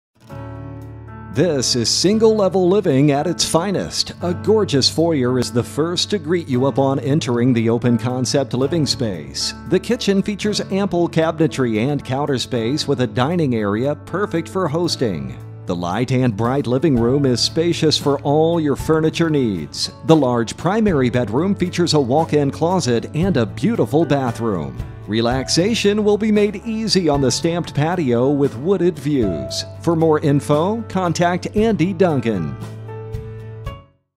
1148 S Red Maple Way Downingtown, PA | ColdwellBankerHomes.com
1148 S Red Maple Way, Downingtown, PA

Here is an opportunity to own a pristine and beautiful home in the award-winning 55+ community of The Woods at Rock Raymond. Step into the gorgeous foyer and enjoy the open concept of the kitchen, dining and living room. The first-floor primary bedroom features walk-in closets and a beautiful bathro